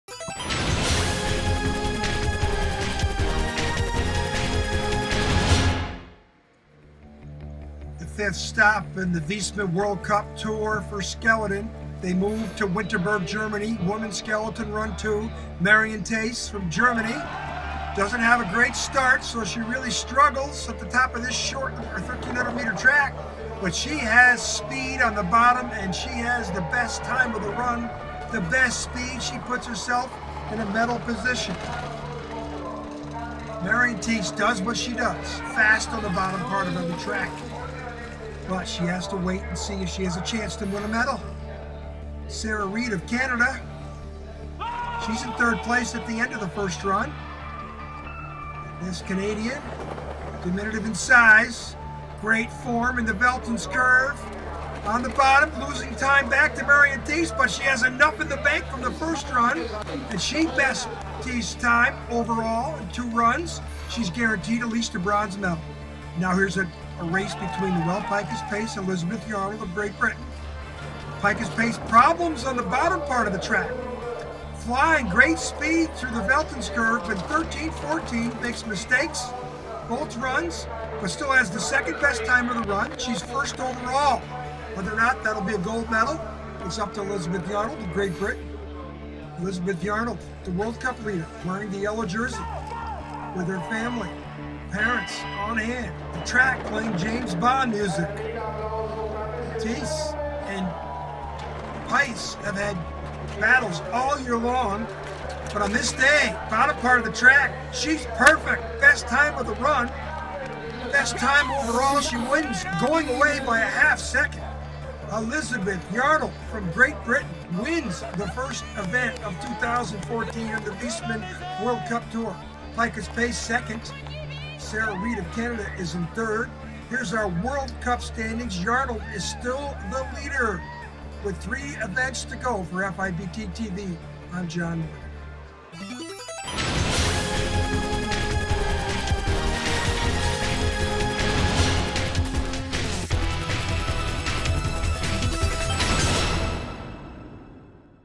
FIBT | Women's Skeleton World Cup 2013/2014 - Winterberg Highlights
2012 World Championship bronze medallist Elizabeth Yarnold from Great Britain outdueled Noelle Pikus-Pace of the United States to win the fourth World Cup event of the season and extend her lead in the overall ranking. Sarah Reid of Canada, who won the bronze medal in the 2013 World Championships, came third in Winterberg.